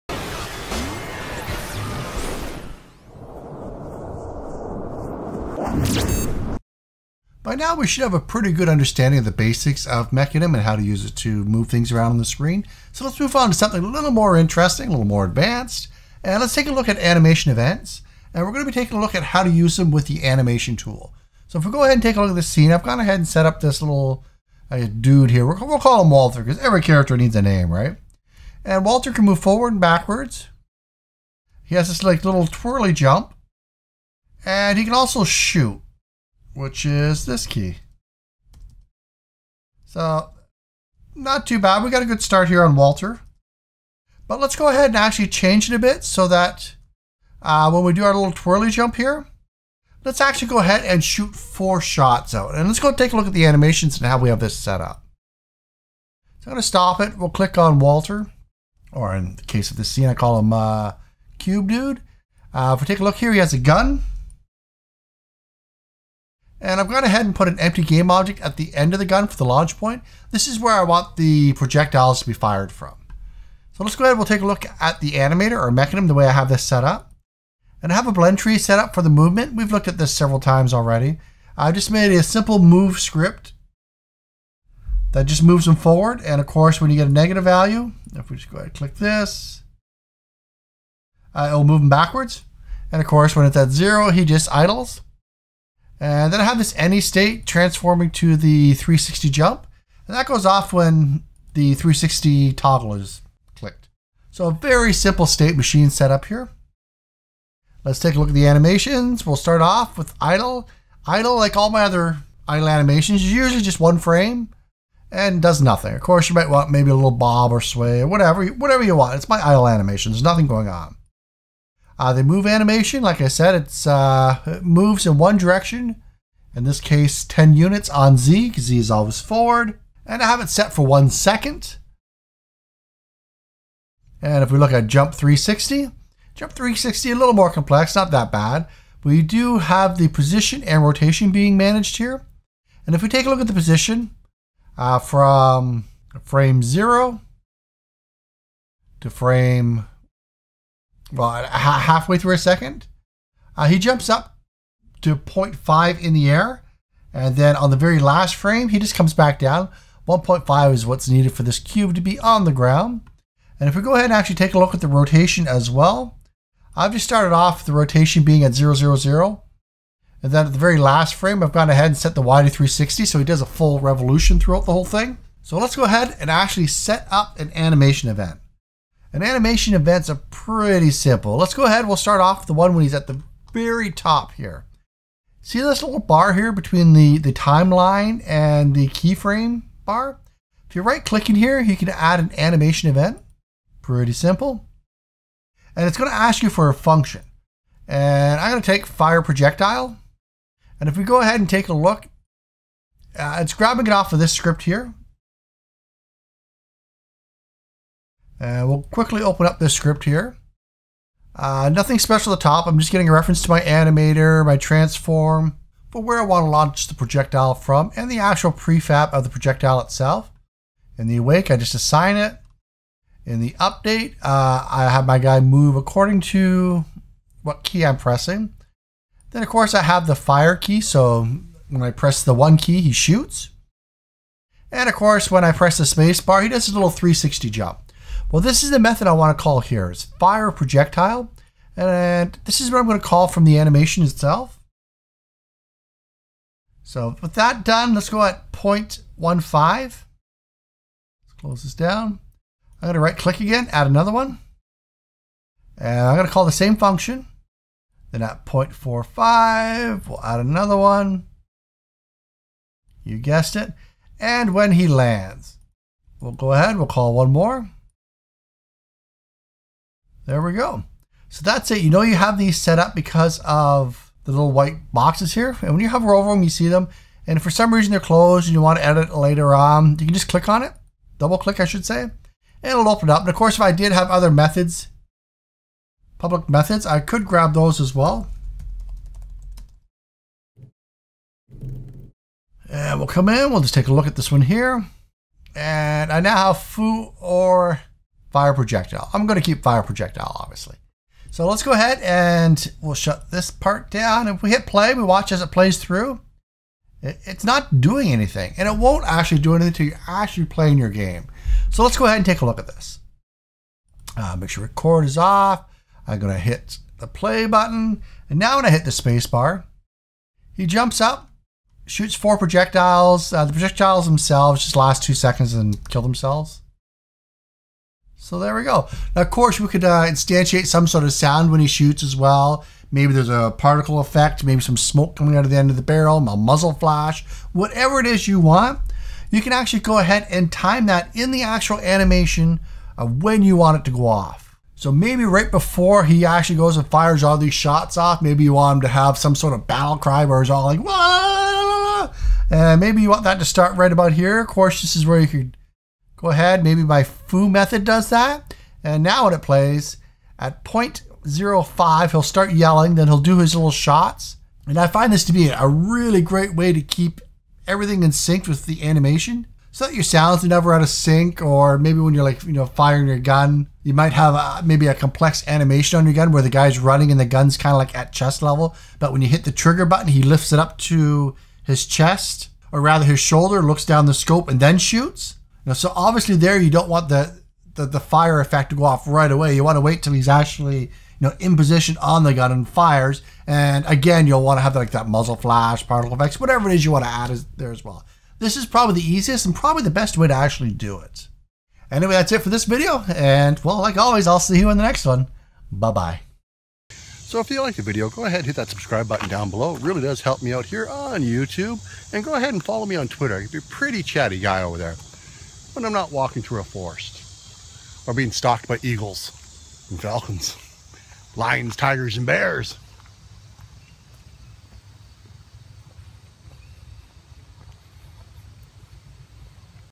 Mecanim: Animation Events With The Animation Tool - Unity 5
Animation Events are about the best way possible to sync up some action with an animation. Here is how to do it using the Animation Tool that comes builtin with Unity 5.